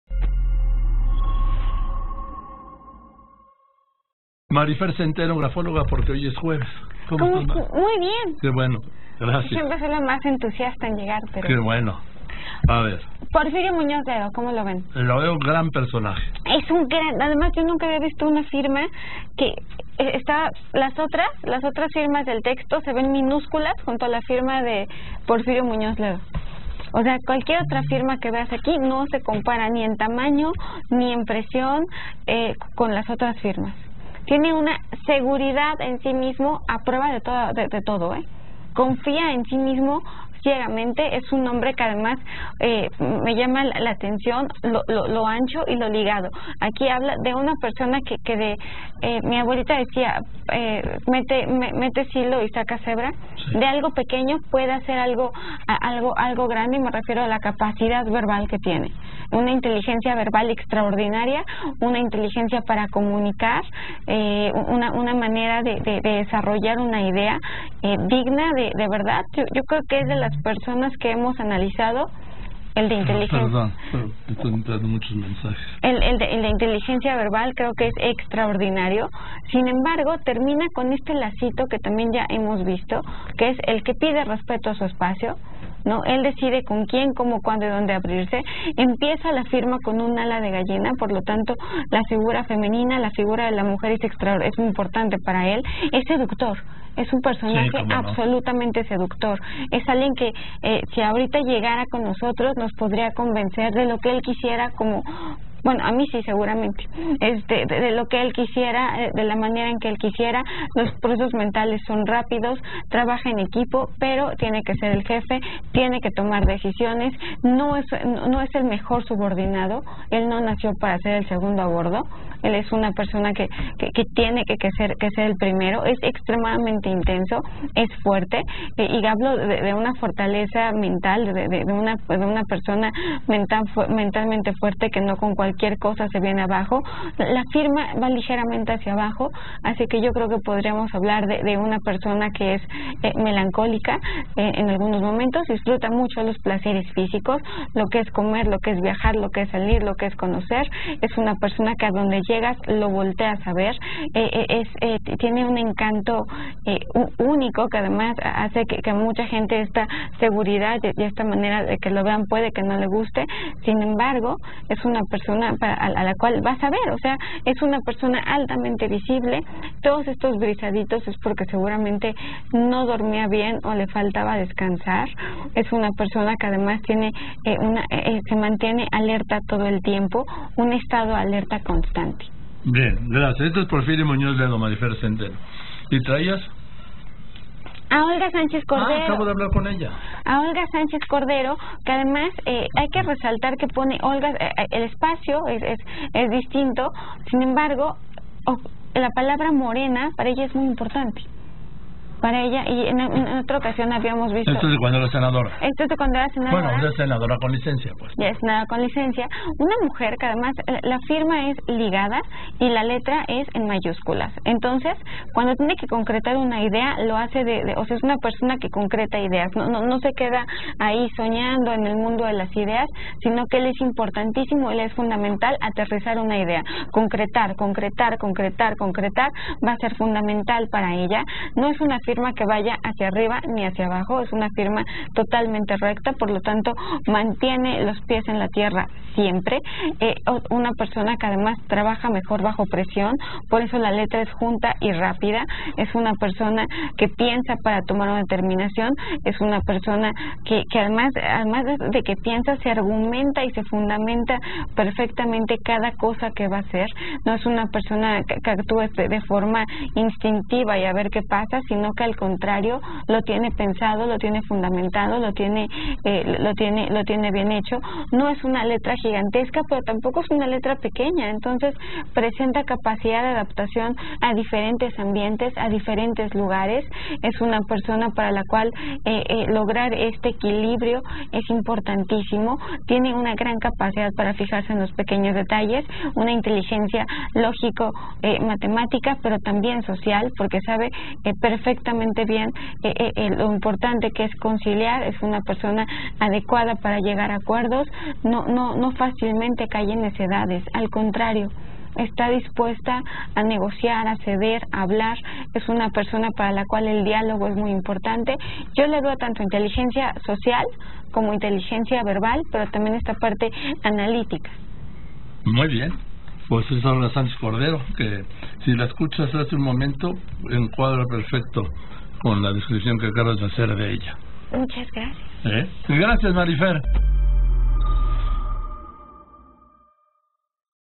Maryfer Centeno analiza las firmas de Muñoz Ledo y Sánchez Cordero
La grafóloga Maryfer Centeno analizó las firmas del presidente de la Cámara de Diputados y de la titular de la Secretaría de Gobernación